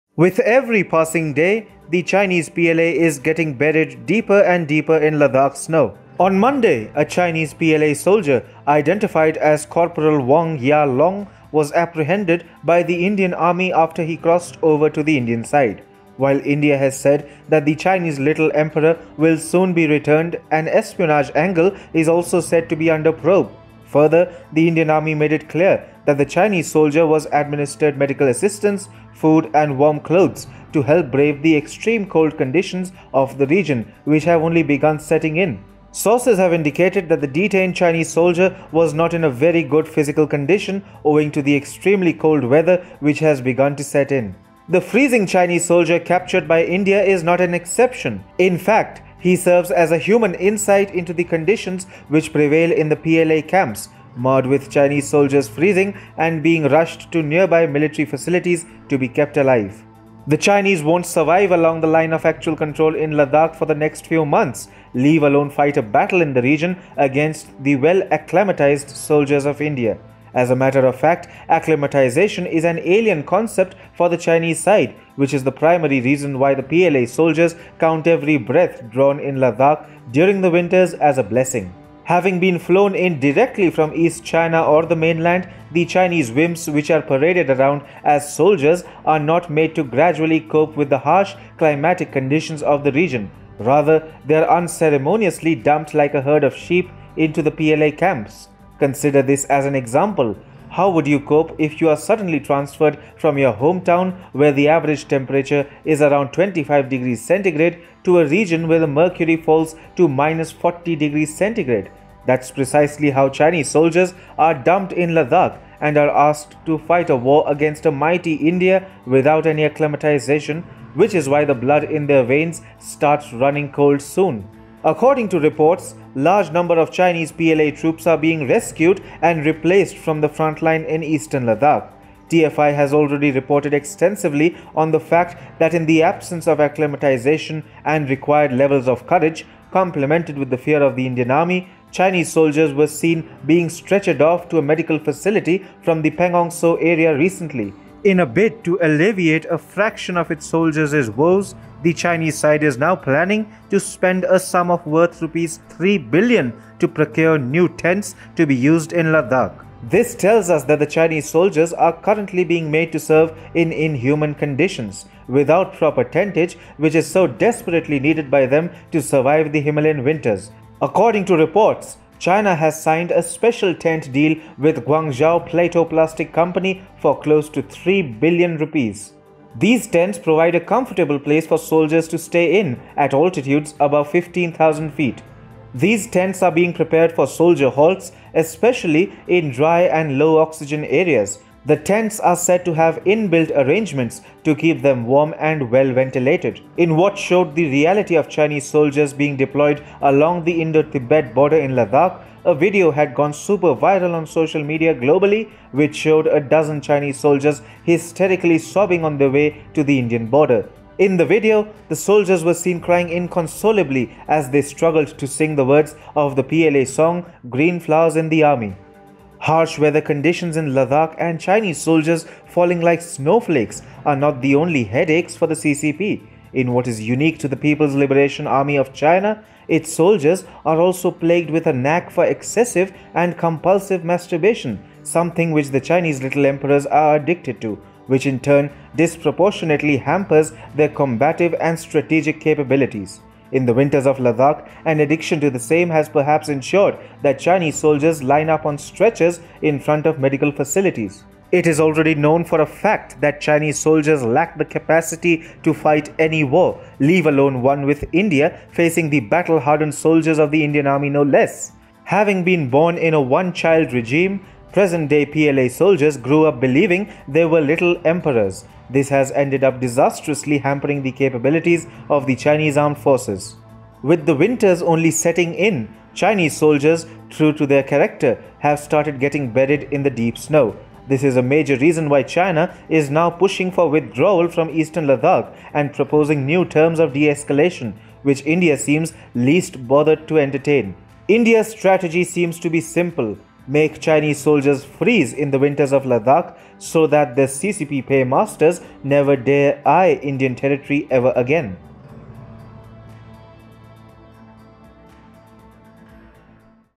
With every passing day, the Chinese PLA is getting buried deeper and deeper in Ladakh snow
The freezing Chinese soldier captured by India is not an exception. In fact, he serves as a human insight into the conditions which prevail in the PLA camps, marred with Chinese soldiers freezing and being rushed to nearby military facilities to be kept alive. Watch the video for more!